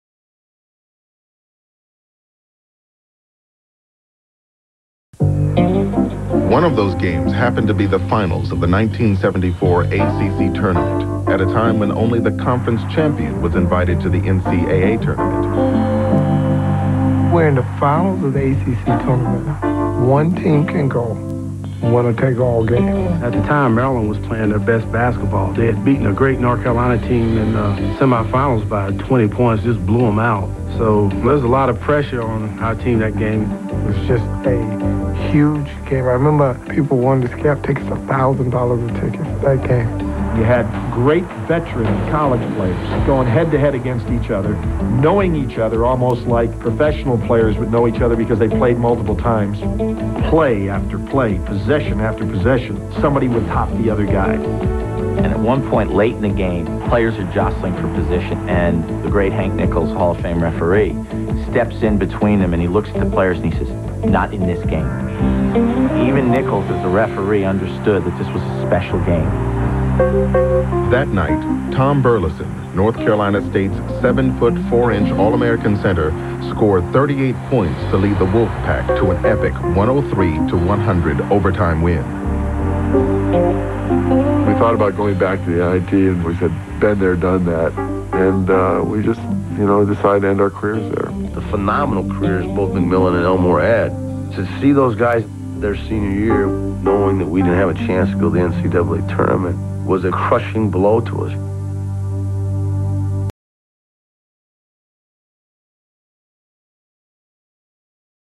1974 ACC Tournament Final
One of the best games ever? Maryland vs. NC State